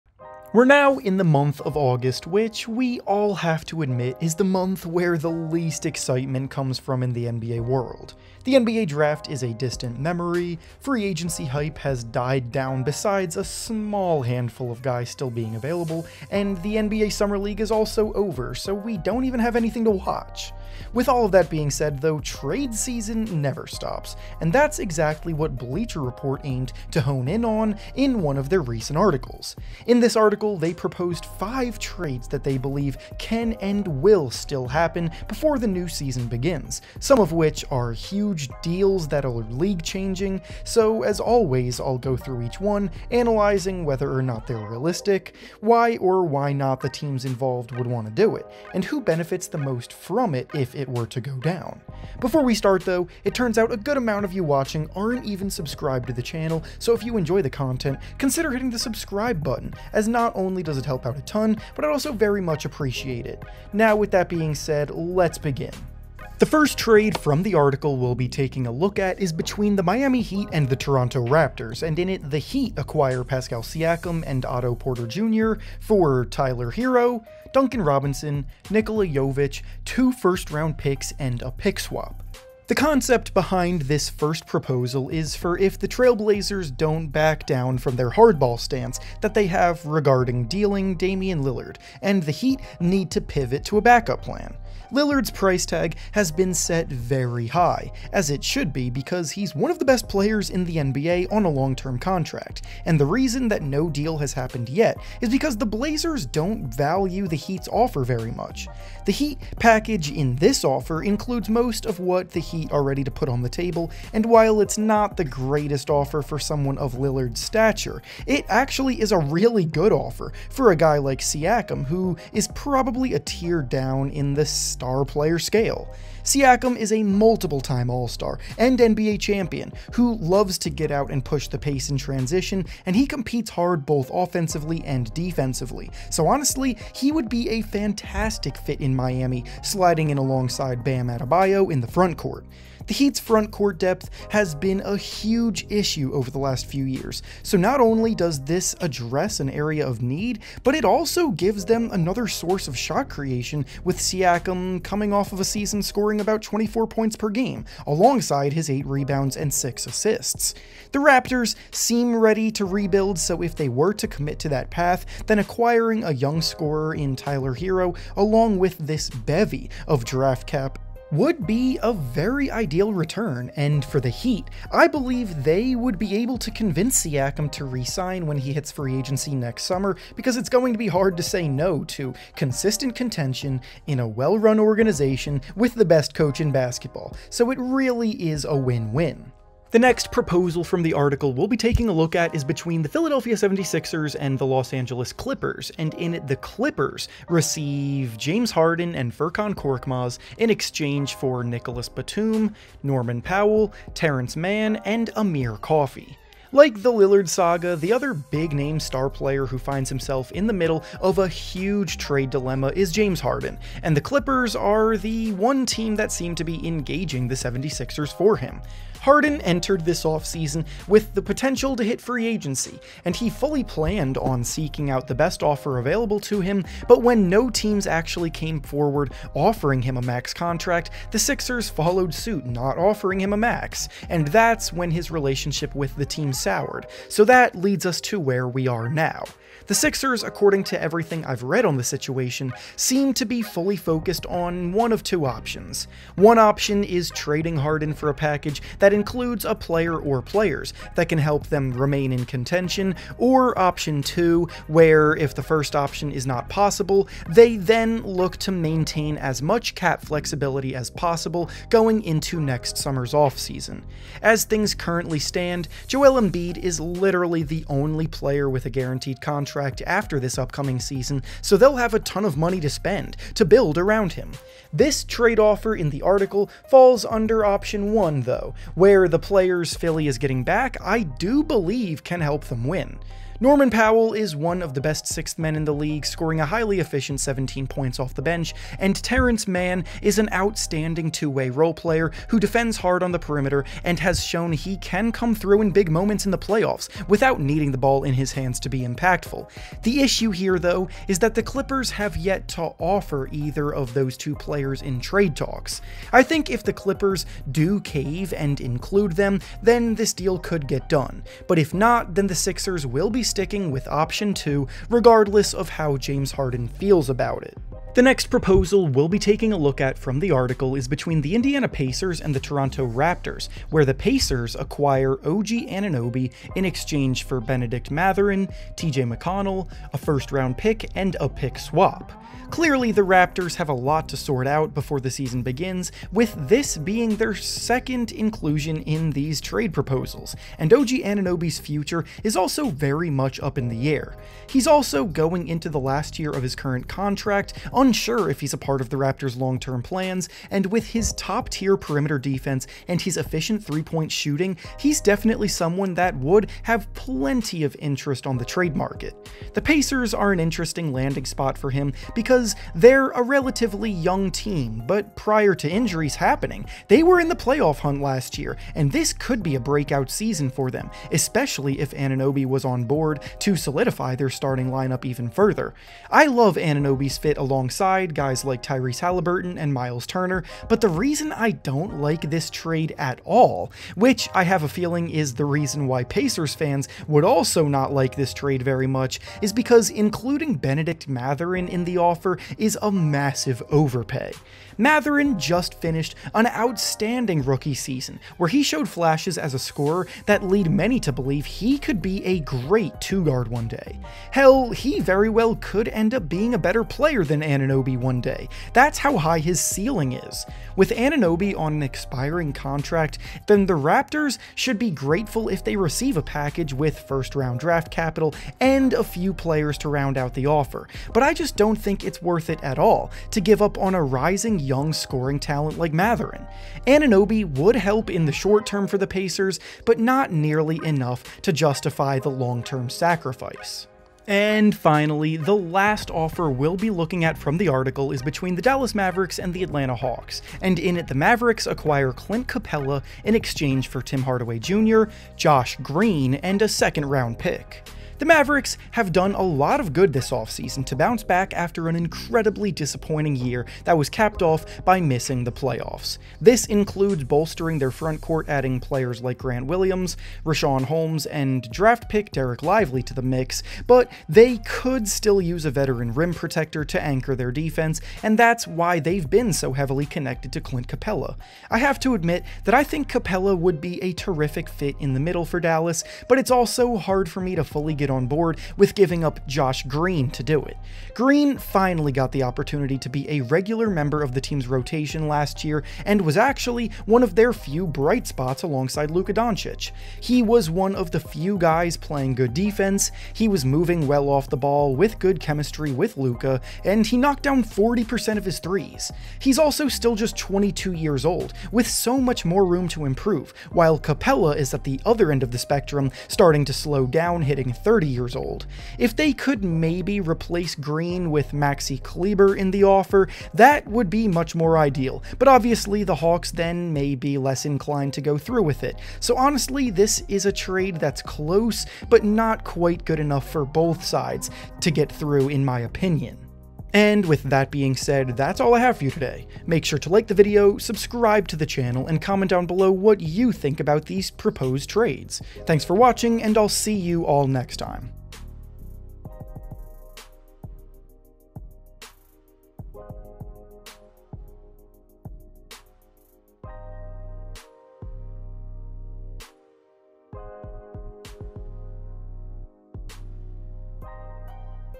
4 Blockbuster NBA Trades That Are ACTUALLY Still Happening This Offseason…
In this video we analyze hypothetical blockbuster NBA trades proposed by Bleacher Report that are still on their way before the start of the 2024 NBA season... Tyler Herro, Pascal Siakam, James Harden and more discussed

SEATGEEK CODE: hoopsreference ($20 off your first purchase at seatgeek.com)